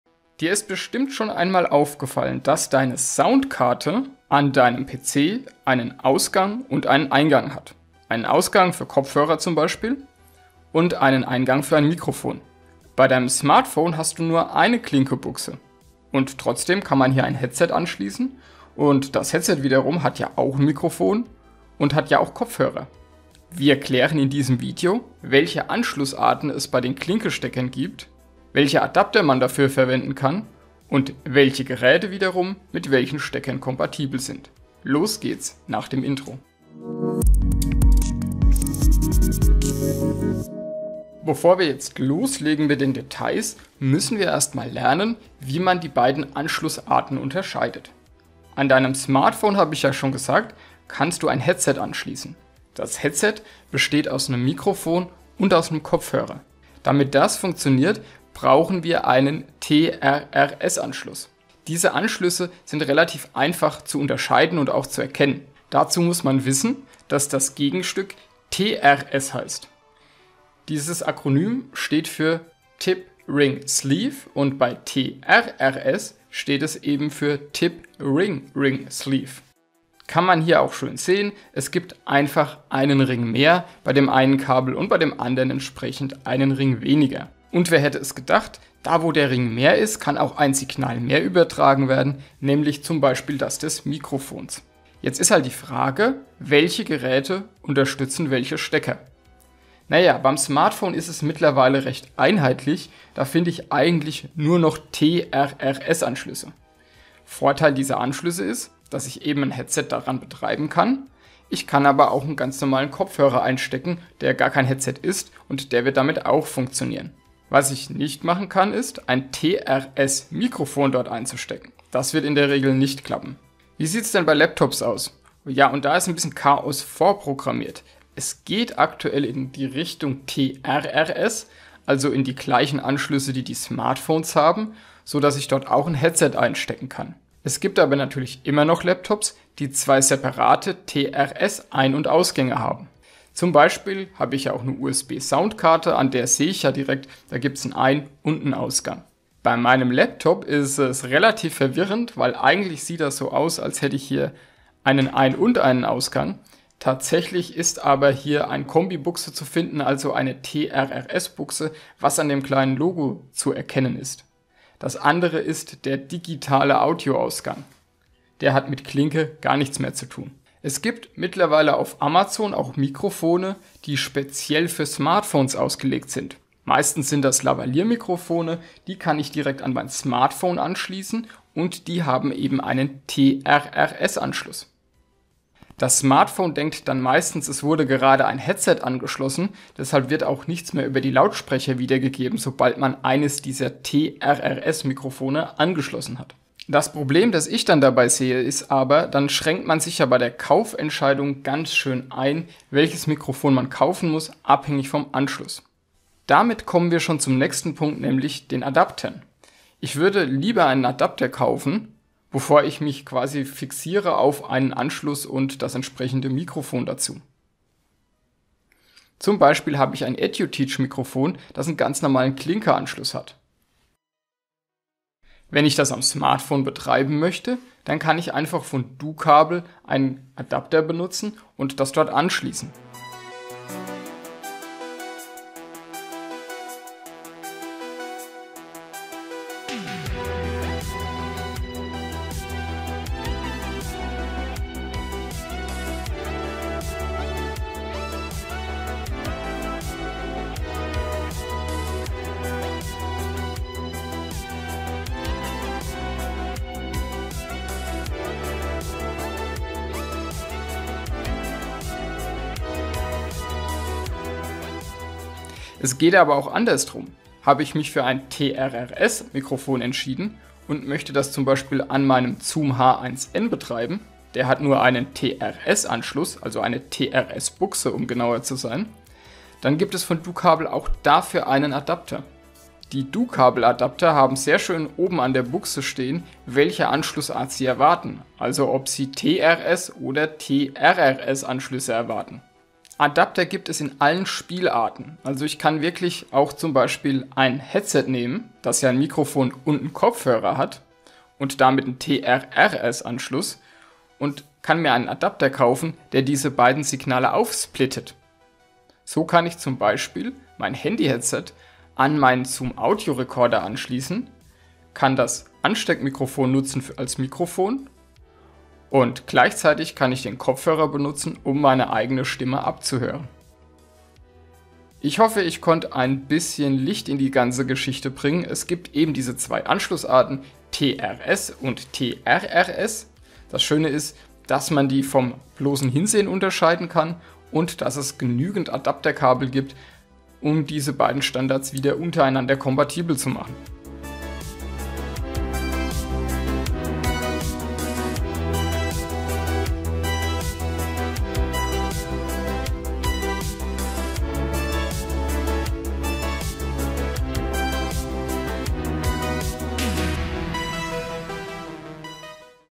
Smartphone Headset 🎧 an PC Soundkarte: Unterschied zwischen TRRS und TRS Anschlüssen erklärt!
DuKabel TRRS auf TRS*
DuKabel TRS auf TRRS*
TRS Mikrofon & TRS Kopfhörer auf TRRS*
TRRS Headset auf 2 TRS Stecker*
00:00 Intro
00:41 TRRS und TRS unterscheiden
01:34 Geräte mit TRRS und TRS
03:16 Adapter mit TRRS und TRS
05:24 Zusammenfassung
Die beiden Adapter für von DuKabel für dieses Video zur Verfügung gestellt. DuKabel hat keinen Einfluss auf das Video genommen.
Das Rode SC3 ist auch ein TRS Adapter